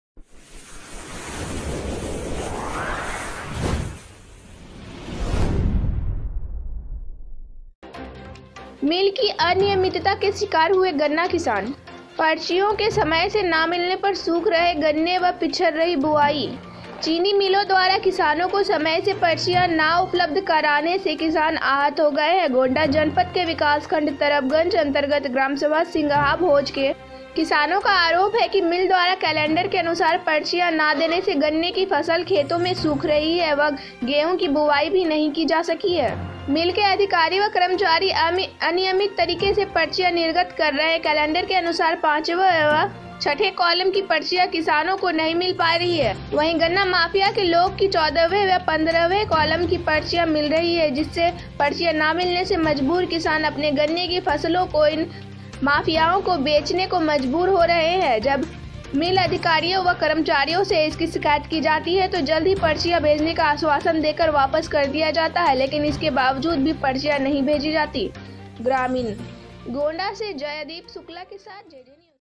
मिल की अनियमितता के शिकार हुए गन्ना किसान,पर्चियों के समय से न मिलने पर |jankari junction
*स्लग-मिल की अनियमितता के शिकार हुए गन्ना किसान*

*पर्चियों के समय से न मिलने पर सूख रहे गन्ने व पिछड़ रही बुवाई*

*रिपोर्टर-जयदीप शुक्ला*

*स्थान-गोण्डा*

*ऐंकर-*
चीनी मिलों द्वारा किसानों को समय से पर्चियां न उपलब्ध कराने से किसान आहत हो गए हैं।
गोण्डा जनपद के विकासखण्ड तरबगंज अन्तर्गत ग्रामसभा सिंगहा भोज के किसानों का आरोप है कि मिल द्वारा कलेंडर के अनुसार पर्चियां न देने से गन्ने की फसल खेतो में सूख रही हैं व गेंहू की बुवाई भी नही की जा सकी है।
मिल के अधिकारी व कर्मचारी अनियमित तरीके से पर्चियां निर्गत कर रहे हैं।कलेंडर के अनुसार पांचवे व छठे कॉलम की पर्चियां किसानों को नही मिल पा रही हैं।वहीं गन्ना माफिया लोगों की 14वें व 15वें कॉलम की पर्चियां मिल रही हैं।जिससे पर्चियां न मिलने से मजबूर किसान अपनी गन्ने की फसलों को इन्ही माफ़ियायो को बेचने को मज़बूर हो रहे हैं।
जब मिल अधिकारियों व कर्मचारियों से इसकी शिकायत की जाती है तो जल्द ही पर्चियां भेजने का आश्वासन देकर वापस कर दिया जाता है।लेकिन इसके बावजूद भी पर्चियाँ नही भेजी जाती।

*बाईट-ग्रामीणों की*